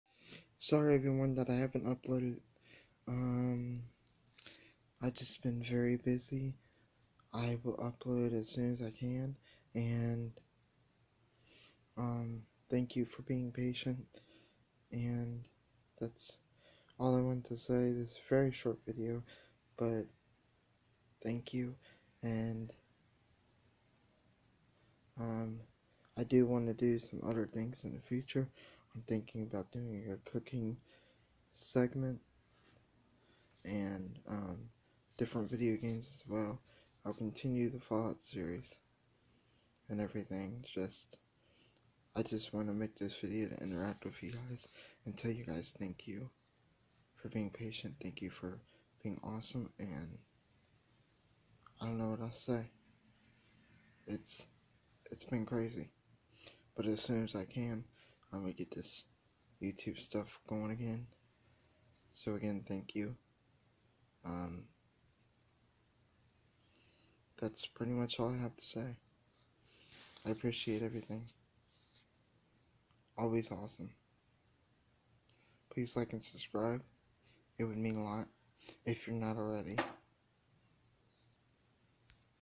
Update 1-15-18 sorry for not uploading
Please like And subscribe comment down below I will respond sorry for not uploading again my phone doesn't have much space but when I'm able to I'll get better equipment and I'll make really good videos for you but thank you for standing by and not giving up on me :)